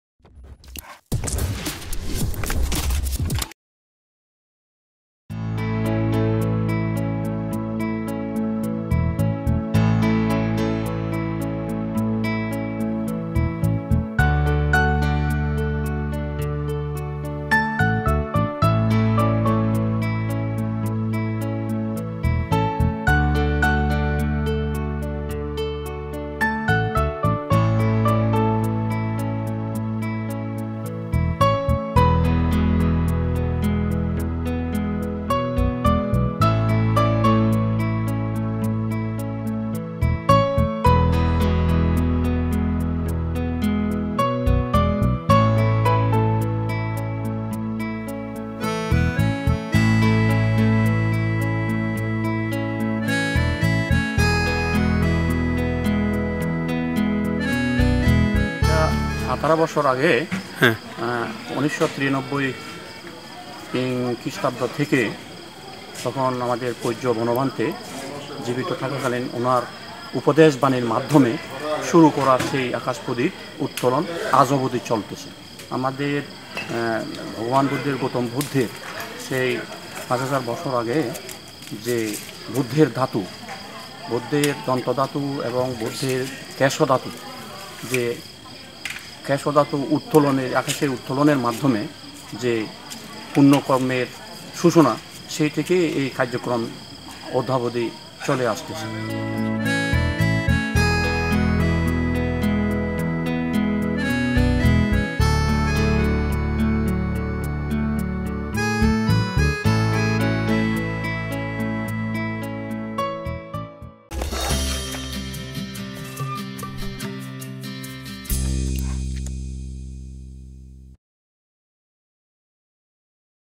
আকাশপ্রদীপ প্রজ্বালন পূজা || Prothom Alo News
আকাশপ্রদীপ প্রজ্বালন-গৌতম বুদ্ধের দন্ত ও কেশ ধাতু আকাশে উত্তোলনের পূর্ণ কর্মের মাধ্যমে সূচনা হয় আকাশপ্রদীপ প্রজ্বালন পূজা। গত ১৮ বছর ধরে রাঙামাটির রাজবন বিহারে এই পূজা অনুষ্ঠিত হয়ে আসছে।স্বর্গ লাভের আশায় ও বিশ্ব শান্তি কমানায় প্রতি বছর এই মন্দিরে সমবেত হয় শতশত বৌদ্ধ ধর্মাবলম্বী লোকজন। একমাস ধরে চলা এই পূজার শেষ দিনেও ছিল হাজারো লোকের ভিড়।